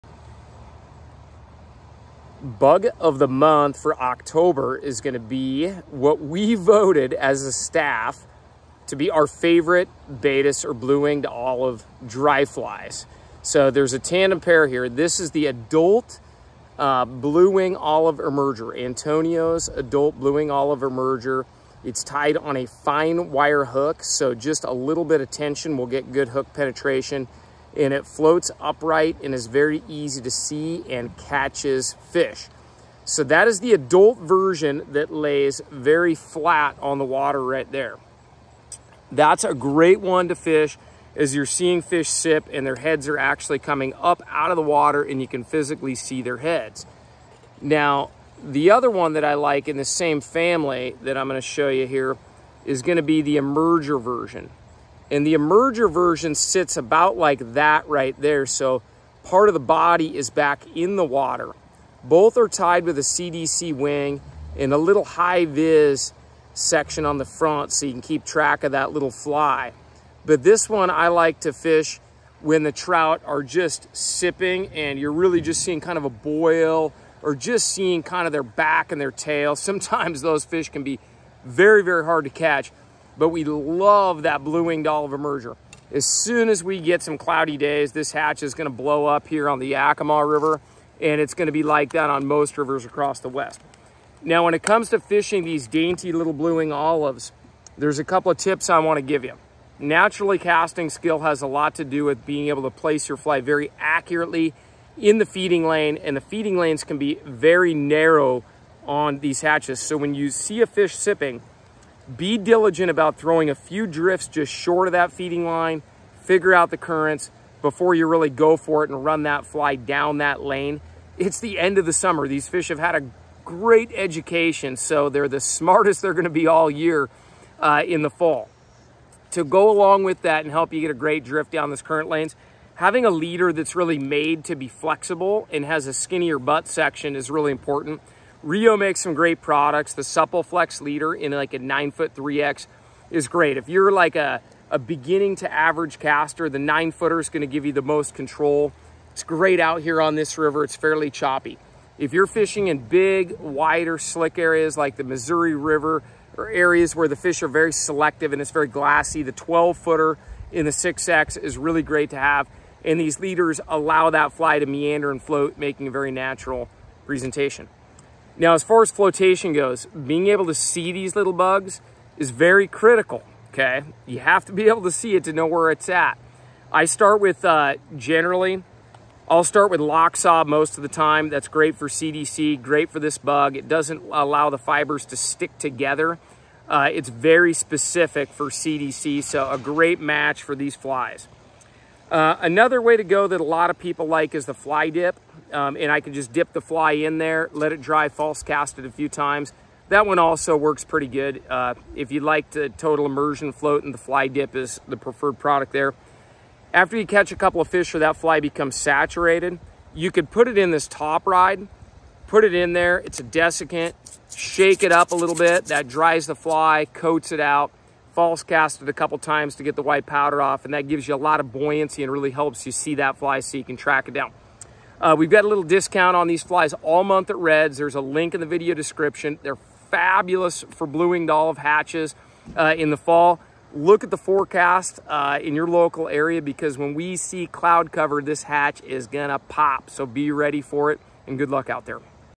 Bug of the Month October // Antonio’s BWO Dry Flies
The Antonio's BWO Adult and Emerger are excellent choices. These were voted "Bug of the Month" by our internal shop staff for October. Having a dry fly you can trust is critical to success.